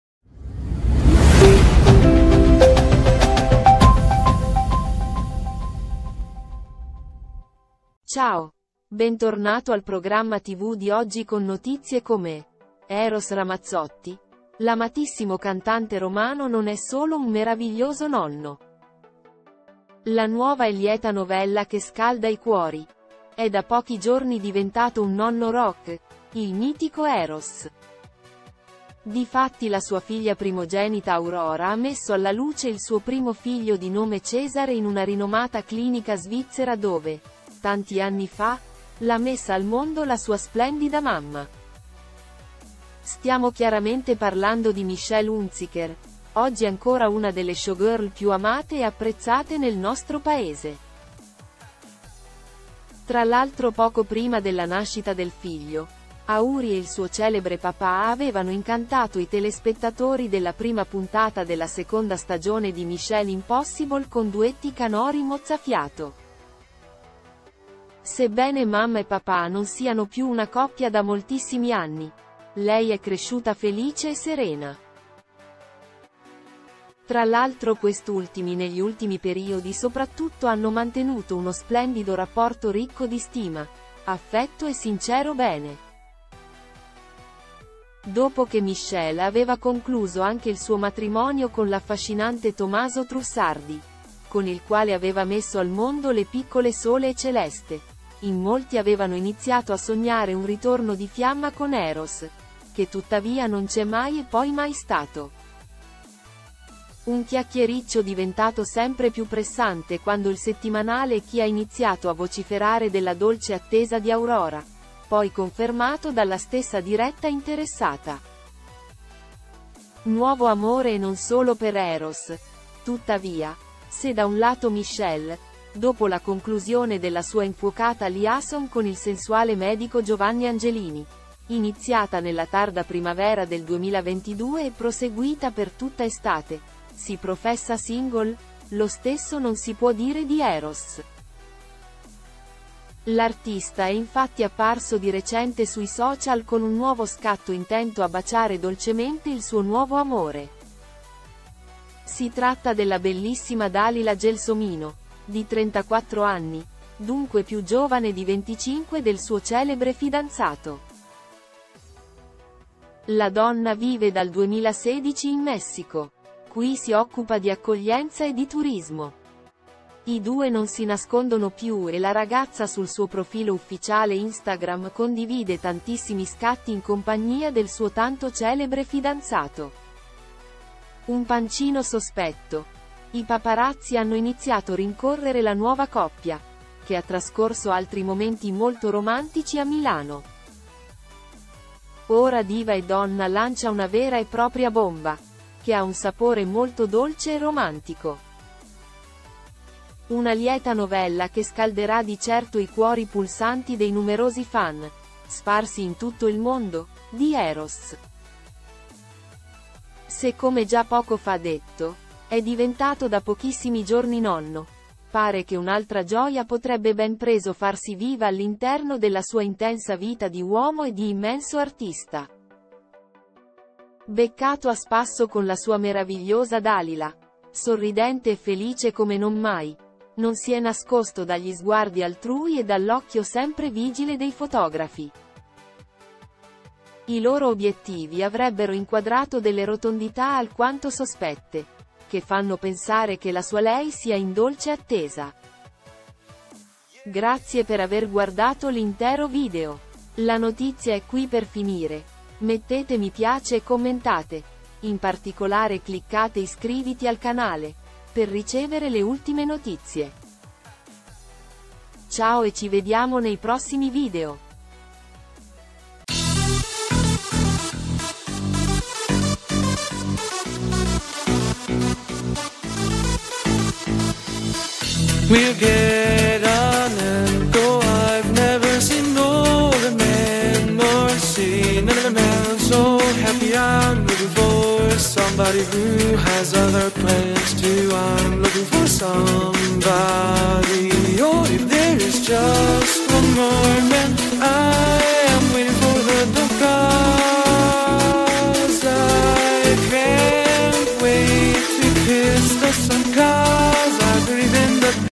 Eros Ramazzotti altro che nonno! La famiglia si allarga ancora e i fan sognano ad occhi aperti
È da pochi giorni diventato un nonno rock, il mitico Eros. Difatti la sua figlia primogenita Aurora ha messo alla luce il suo primo figlio di nome Cesare in una rinomata clinica svizzera dove, tanti anni fa, l’ha messa al mondo la sua splendida mamma. Stiamo chiaramente parlando di Michelle Hunziker, oggi ancora una delle showgirl più amate e apprezzate nel nostro Paese.

Tra l’altro poco prima della nascita del figlio, Aury e il suo celebre papà avevano incantato i telespettatori della prima puntata della seconda stagione di Michelle Impossible con duetti canori mozzafiato. Sebbene mamma e papà non siano più una coppia da moltissimi anni, lei è cresciuta felice e serena. Tra l’altro quest’ultimi negli ultimi periodi soprattutto hanno mantenuto uno splendido rapporto ricco di stima, affetto e sincero bene.